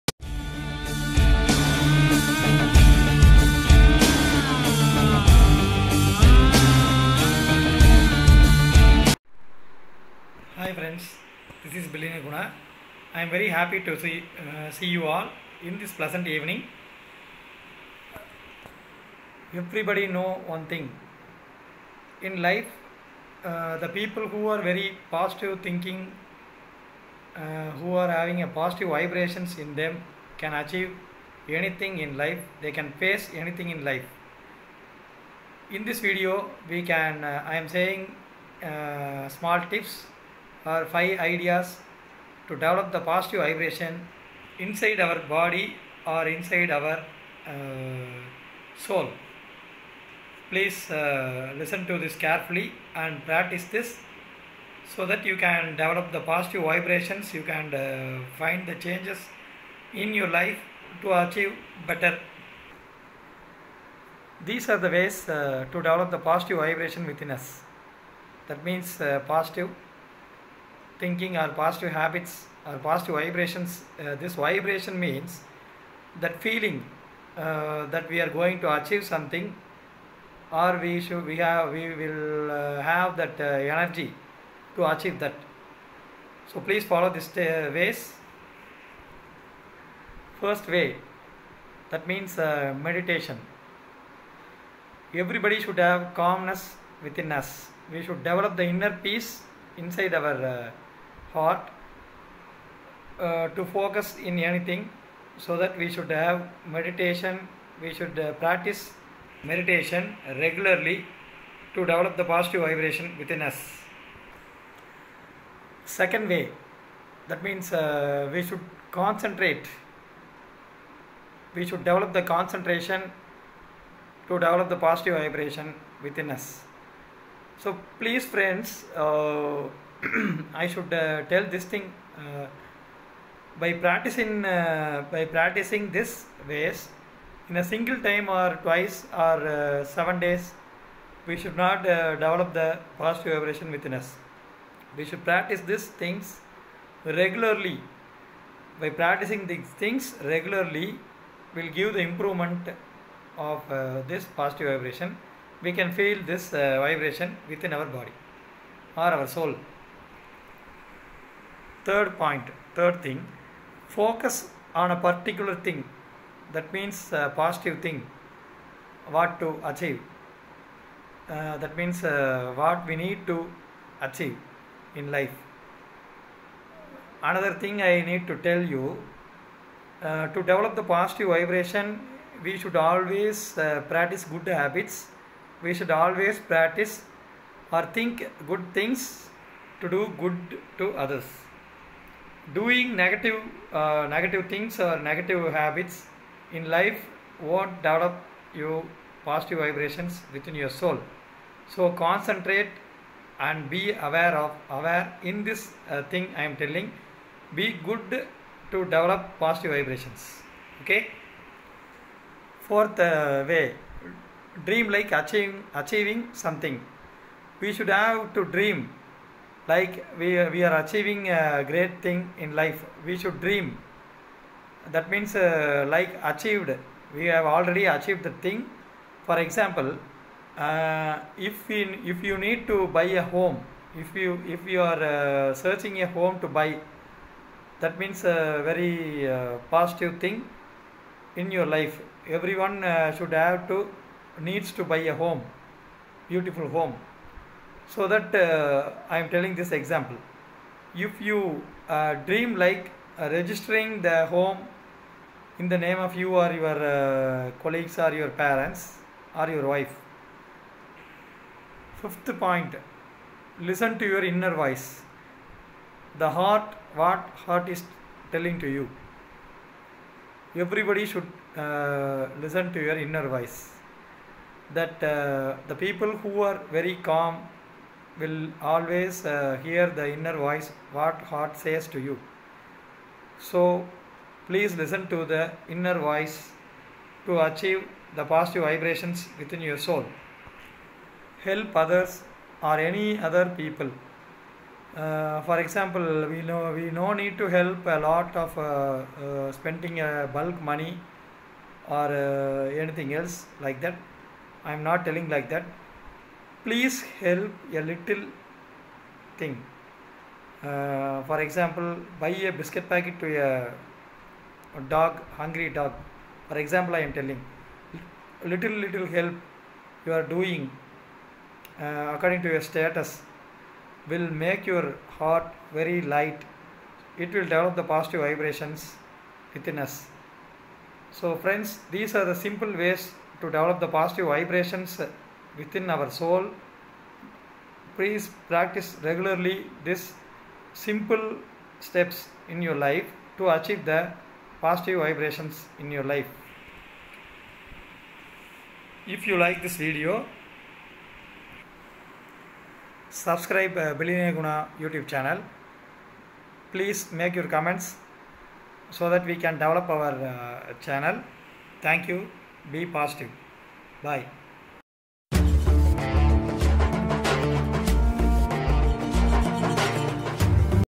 6 WAYS TO DEVELOPE POSITIVE VIBRATION WITHIN US !!!
If we have a positive vibration we can acheive in anything in life.. in this video  we explain about the ways to develope positiveness within us..